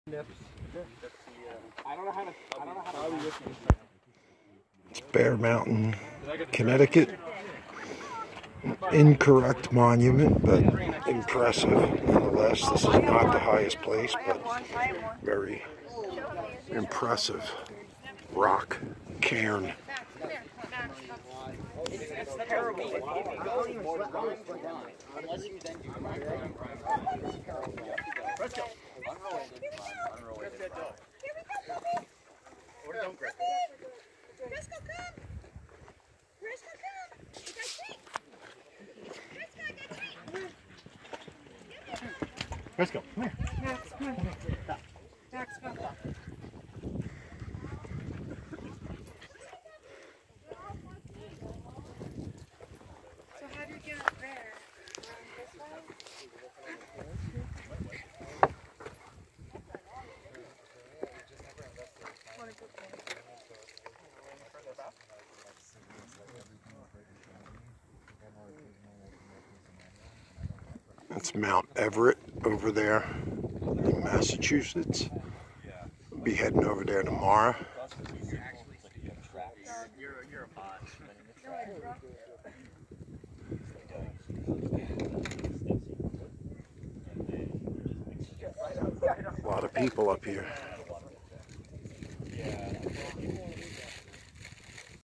Bear Mountain Connecticut
May 2019 A.T. Hike Megalithic Rock Cairn Monument at summit.